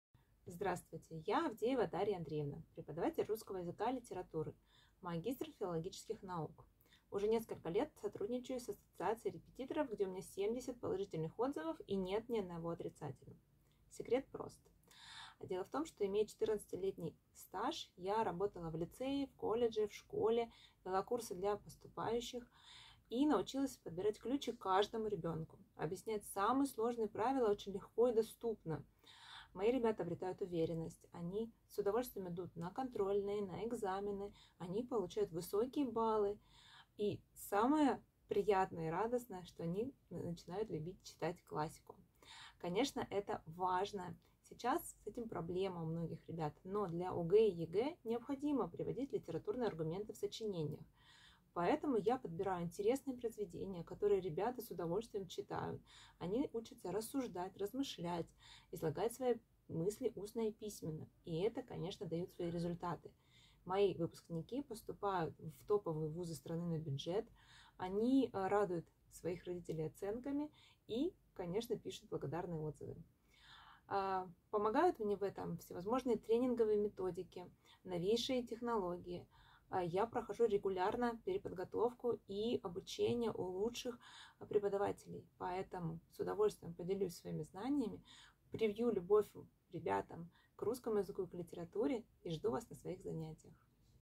Авдеева Дарья Андреевна - репетитор по русскому языку - видеопрезентация для Repetit.ru
Видеопрезентация репетитора по русскому языку и литературе Авдеевой Дарьи Андреевны для REPETIT.RU.

Авдеева Дарья Андреевна окончила Южный федеральный университет.